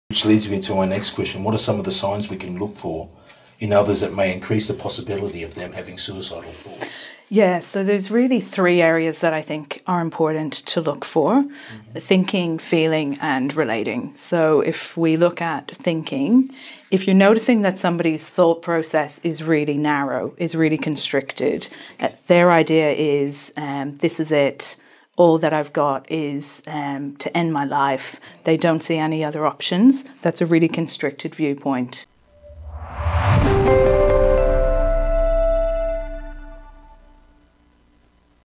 What are the signs we can look for?
Derek Best interviews Eimear Quigly who is a Clinical Psychologist and Senior Lecturer at Edith Cowan University. She is the Director of the Psychology Training Clinic where post-graduate psychology students complete their first placement. She lectures in the areas of mental health assessment and skills training, which includes assessment of suicide and self-harm.
Eimear has worked in the mental health area for 20 years and as a clinical psychologist for the last 13 years. She has worked across government, not-for-profit and private industries in Perth and the Kimberley region of Western Australia.
Eimear’s passion is supporting people who are suffering and to use her skills and knowledge to improve the wellbeing of as many people as possible. She believes in the power of connection as the antidote too suffering.